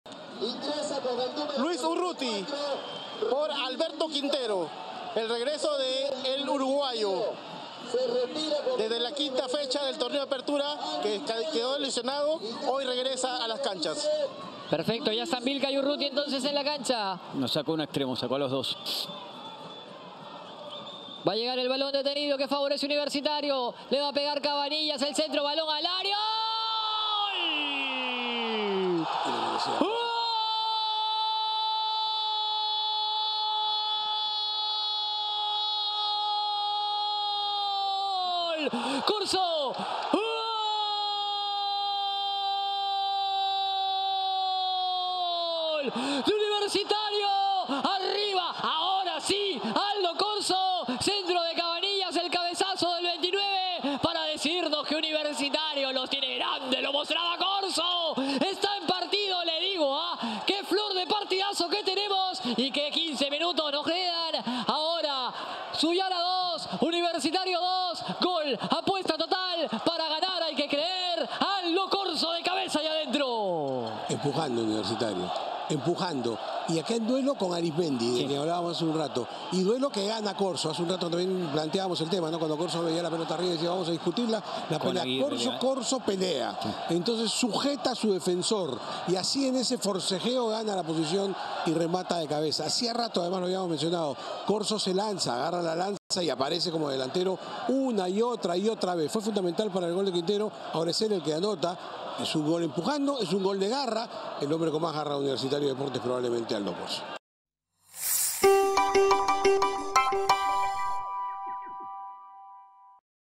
Liga1 Betsson: Aldo Corzo anotó así el Alianza Atlético 2-2 Universitario de Deportes
¡QUÉ PARTIDAZO! Ingresando Luis Urruti ⚽ en Universitario 🟠, Aldo Corzo 🔥 cabeceó el balón y empató el partido.